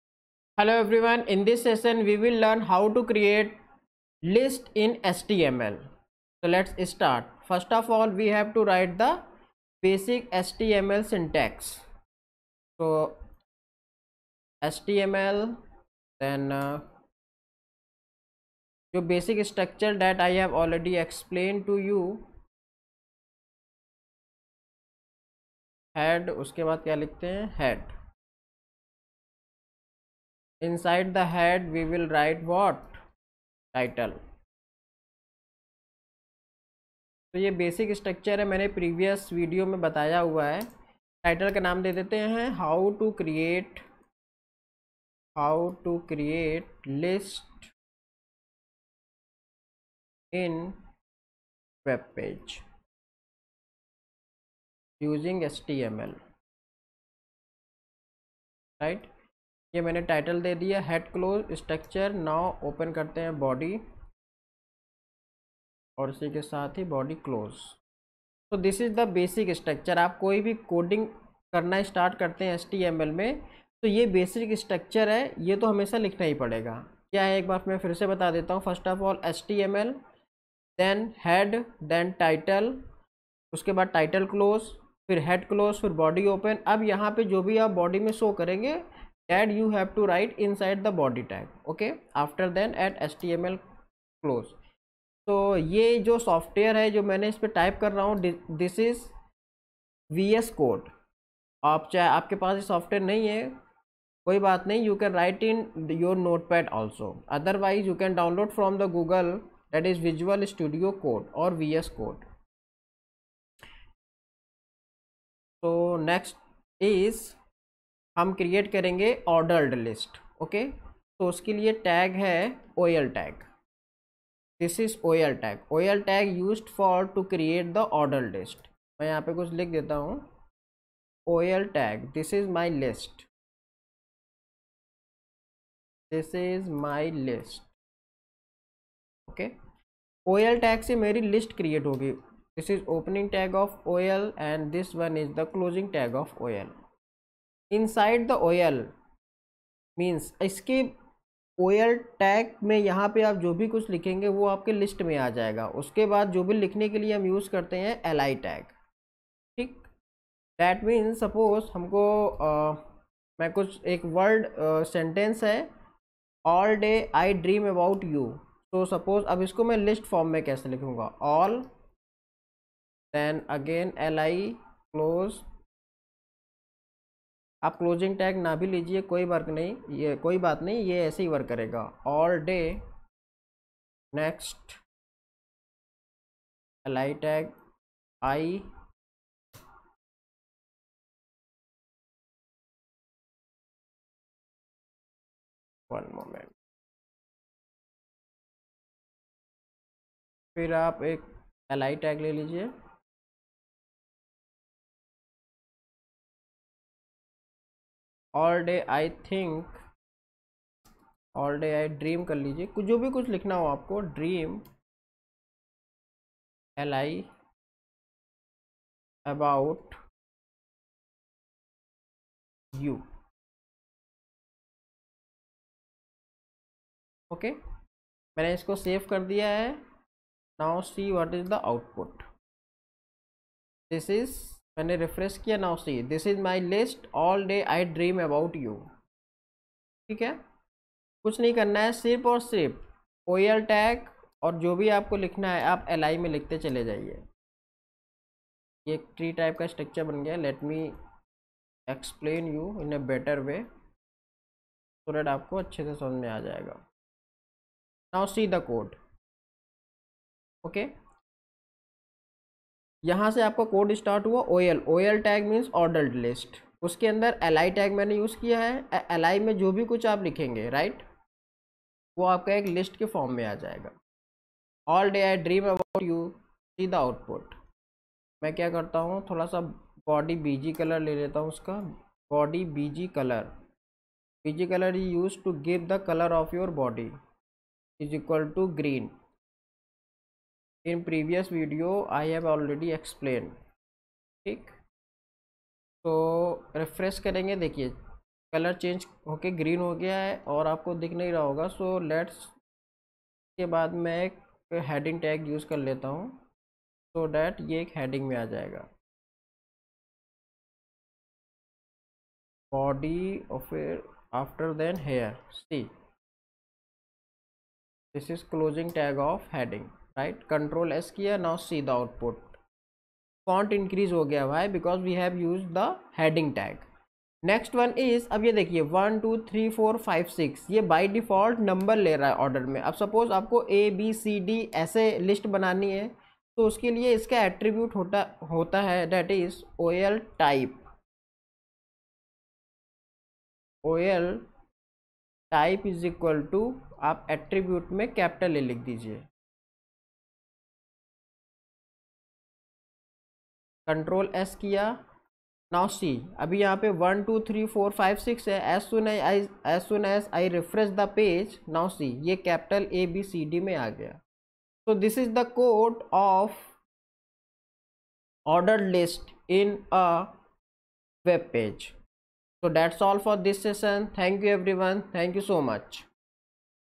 TO CREATE LIST IN HTML || LIST VIEW || LIST DESIGN || HTML LIST TUTORIAL
IN THIS VIDEO you will learn about HOW TO CREATE LIST IN HTML
ordered list

body color
top margin
list in html